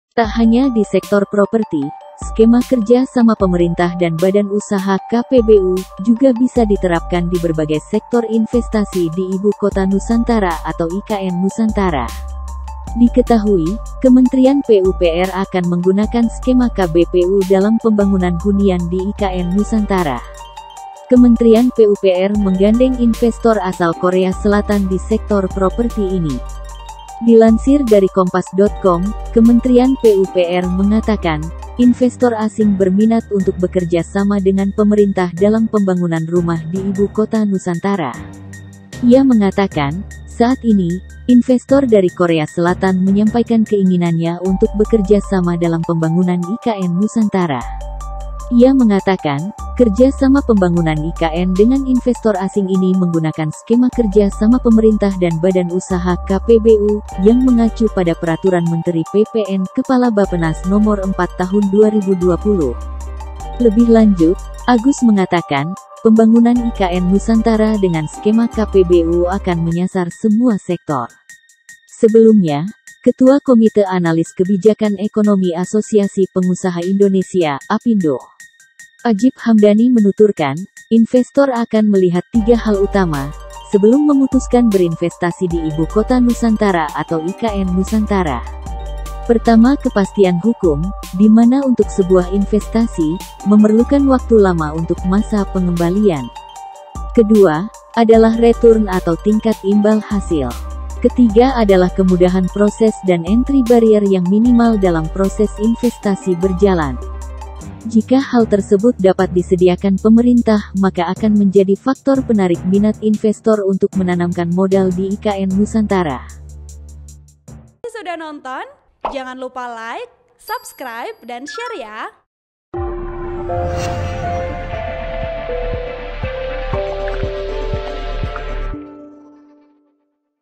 Tak Hanya Properti, Skema KPBU Bisa Diterapkan Pada Investasi Lain di IKN Nusantara
Tak hanya di sektor properti, skema Kerja sama Pemerintah dan Badan Usaha ( KPBU) juga bisa diterapkan di berbagai sektor investasi di Ibu Kota Nusantara atau IKN Nusantara.

Diketahui, Kementrian PUPR akan menggunakan skema KBPU dalam pembangunan hunian di IKN Nusantara.

Kementrian PUPR menggandeng investor asal Korea Selatan di sektor properti ini.

Dilansir dari Kompas.com, Kementrian PUPR mengatakan, investor asing berminat untuk bekerja sama dengan pemerintah dalam pembangunan rumah di Ibu Kota Nusantara.

Ia mengatakan, saat ini, investor dari Korea Selatan menyampaikan keinginannya untuk bekerja sama dalam pembangunan IKN Nusantara.

Editor: Robin